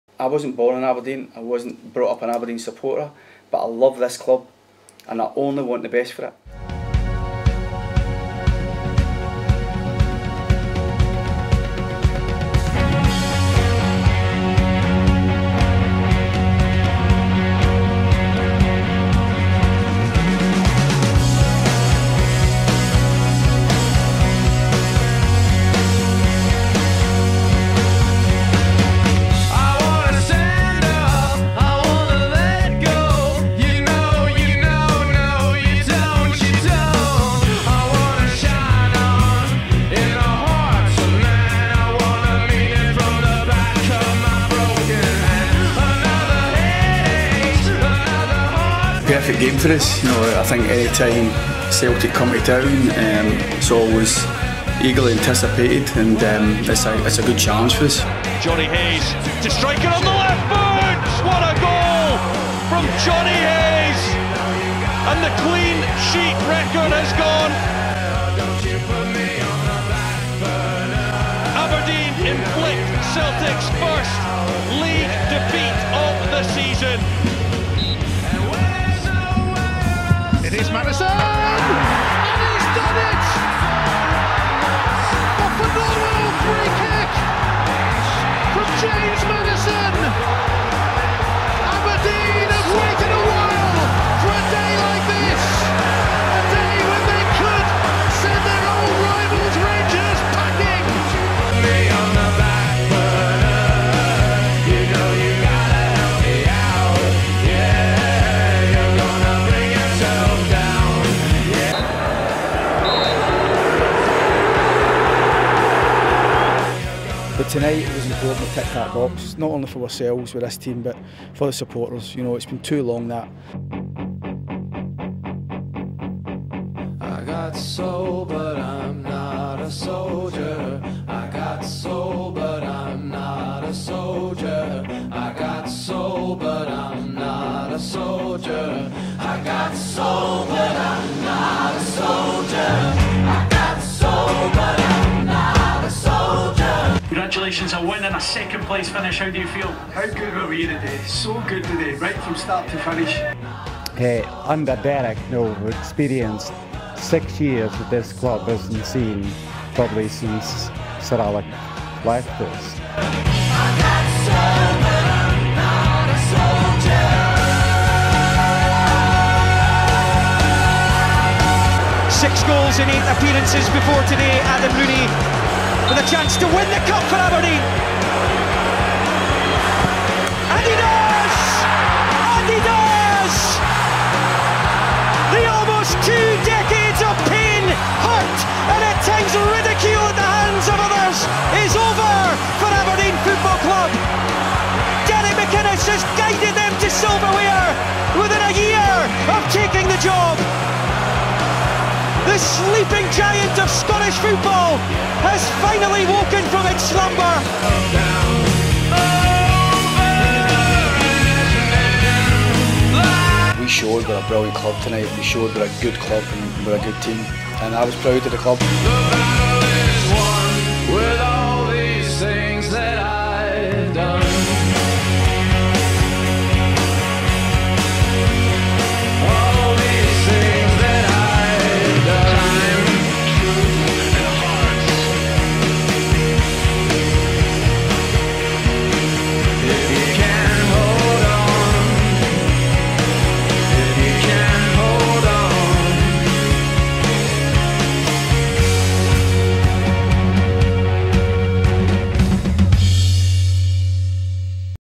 The Derek McInnes Years - Aberdeen FC
A look back at some of the most memorable moments of Derek McInnes' time at Aberdeen Football Club. For many, including myself, the manager delivered the first trophy that AFC have won in my lifetime. There were plenty of games and goals that stood out, but not all of them made it into this video.

The materials used in this video are primarily the property of Aberdeen Football Club through their RedTV and YouTube channels, but I also acknowledge and give credit to BBC Scotland and Sky Sports.